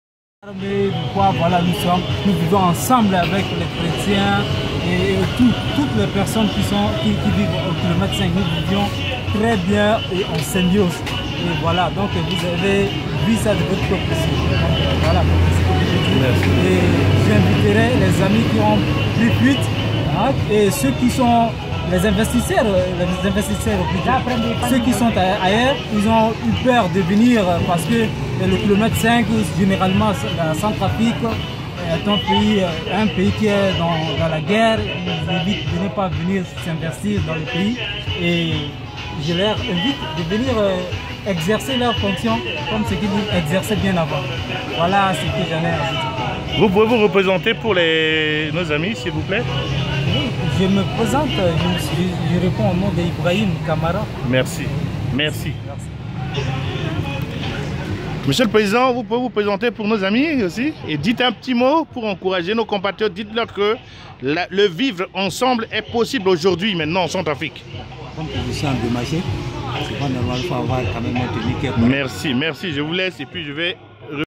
Méssage de Paix par un copatriote Centrafricain de conféssion Musulmane
Message important pour un retour à la cohésion sociale entre filles et fils de la Centrafrique par ce frère qui habite la mosquée centrale au km5 de Bangui où je me suis rendu pour un moment de partage et j'ai pris du thé avec ces frères,ce fut un moment pleins de réjouissance,car pour la plupart de ces déplacés,ce sont des amis avec qui on partageais tout avant les événements tragiques qui allaient nous poussés au pire!